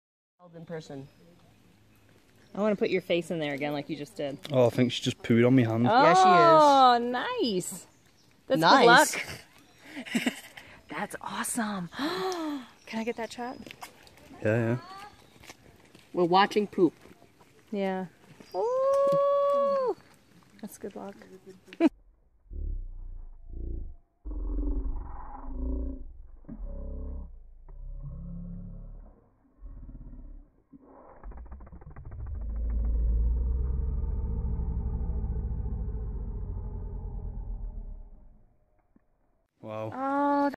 AMAZING SLOW MOTION TAKE OFF OF A BLUE FLOWER BEETLE
I was lucky enough to experience the Doi Inthanon National Park and this was one of the highlights.  A beautiful, female blue flower beetle in the cloud forest.  The King of Thailand has made certain parts of this tropical forest very accessible and it's truly spectacular.  Talk about making you feel alive!

I'm a family friendly blogger who loves food, travel, coffee and culture.